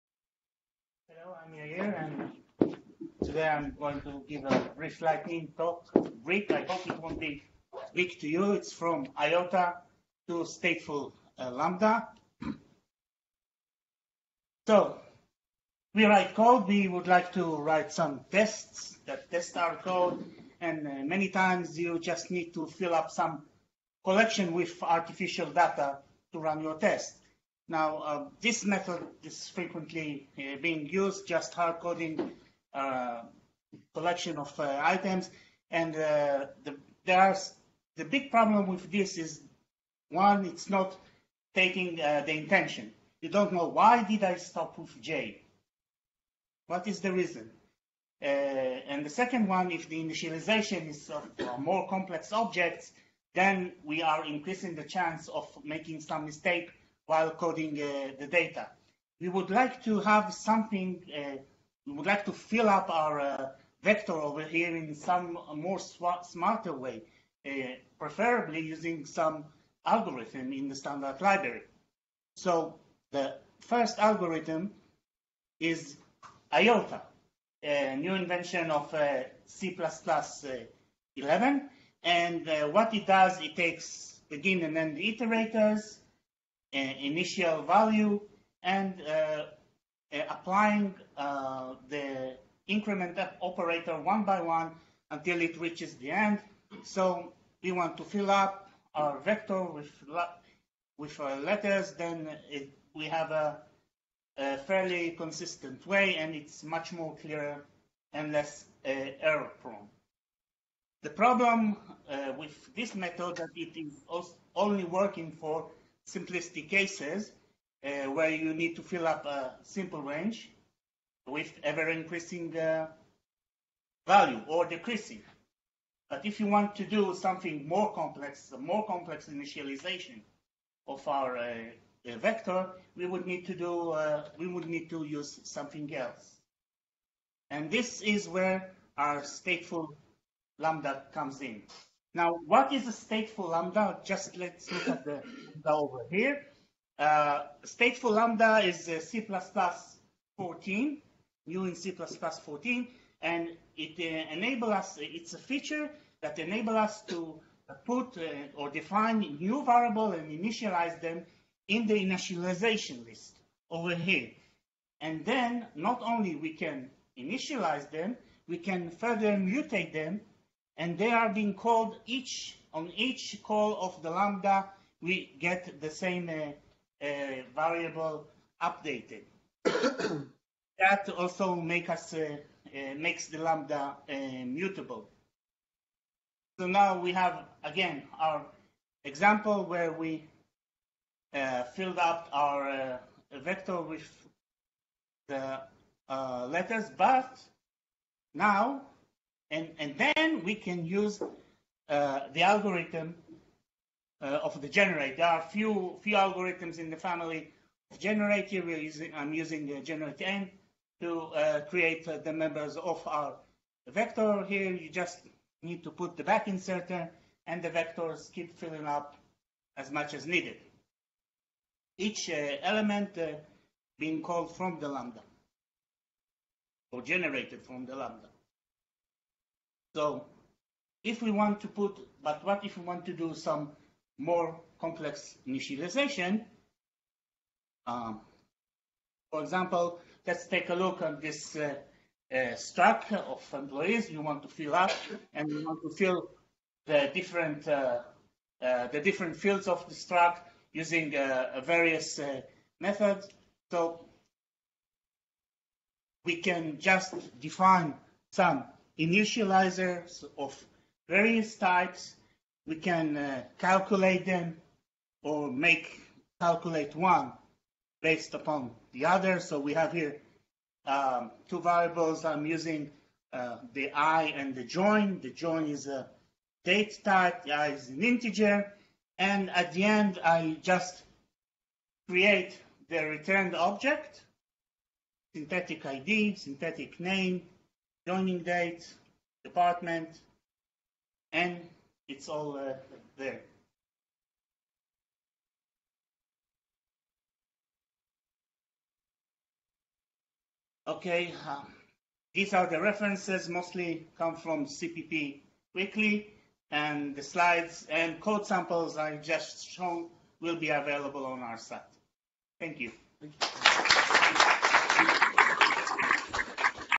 From ι to stateful λ
Stateful lambdas are small lambda constructions that carry states between invocation. I will show some usefulness in creating unit test with them.

*Yair Friedman*
Yair is a secure software professional at Intel Jerusalem. He is also modern C++ evangelist and have been coding in C++ for 27 years. Yair is also a Monty Python fan and always expects the Spanish inquisition.

Presented at Distributed C++ meetup on Oct 25th, 2018 in ShellTech Modiin